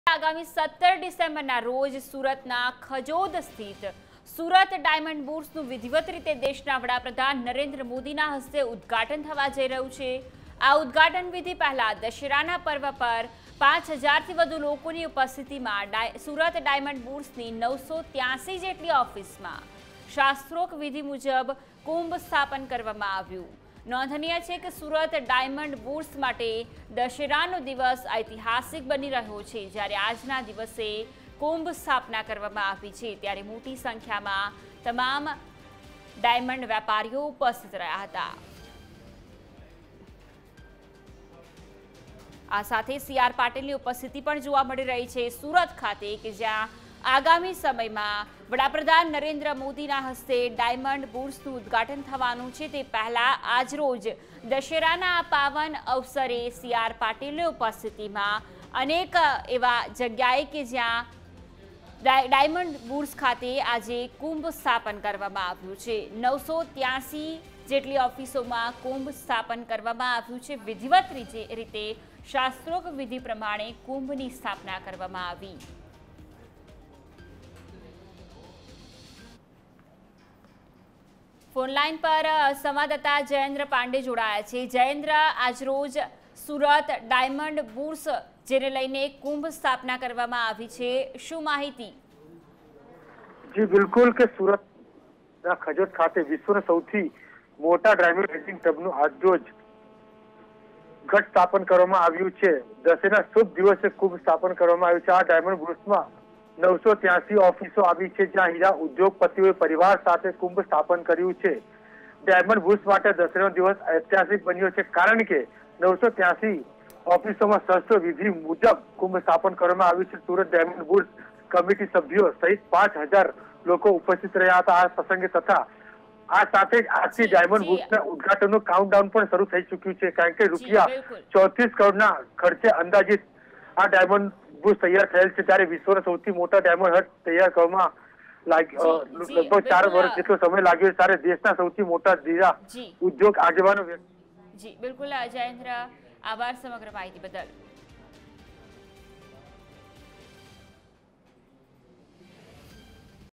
PM મોદી 17મી ડિસેમ્બરે સુરત ડાયમંડ બુર્સનું ઉદ્ઘાટન કરશે
આગામી 17 ડિસેમ્બરના રોજ સુરતના ખજોદ સ્થિત સુરત ડાયમંડ બુર્સનું વિધિવત રીતે દેશના વડાપ્રધાન નરેન્દ્ર મોદીના હસ્તે ઉદ્ઘાટન થવા જઈ રહ્યું છે... આ ઉદ્ઘાટન વિધિ પહેલા દશેરાના પર્વ પર પાંચ હજારથી વધુ લોકોની ઉપસ્થિતિમાં સુરત ડાયમંડ બુર્સની 983 જેટલી ઓફિસમાં શાસ્ત્રોક્ત વિધિ મુજબ કુંભ સ્થાપન કરવામાં આવ્યું. નોંધનીય છે કે, સુરત ડાયમંડ બુર્સ માટે દશેરાનો દિવસ ઐતિહાસિક બન્યો છે...